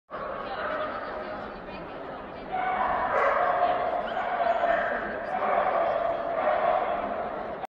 Moscow dream alex-pokoritel serdec Puppy show
Puppy show in Finland
Moscow Dream Alex - pokoritel serdec
(Snezhnyi Dozor Oranienbaum x Reata's Pipi Junior)
Old English sheepdog bobtail
Староанглийская овчарка